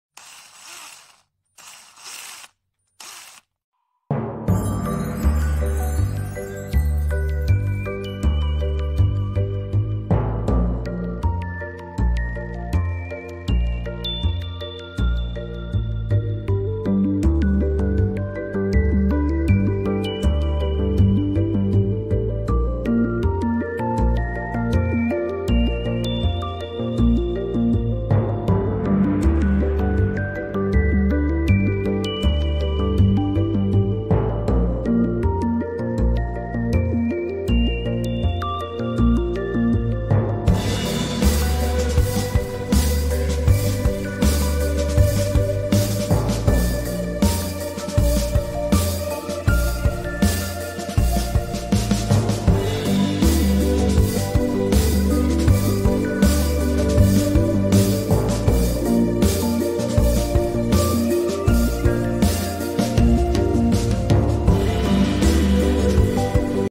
Science and technology - series video No.55
9 Science and technology short video collection...
1. Tetris creative table
2. I want to know what this deformation is for.
3. High-tech automatic page turning machine
4. Wow!
5. The picture that the scientist first captured the laser flying in the air
6. This design is good, no longer worry about the socket is not enough in the future.
7. The magical Ems room, who can tell what is going on?
8. No longer have to worry about being slammed by the other side.
9. Now that technology is getting more and more powerful, robots are riding their own bicycles.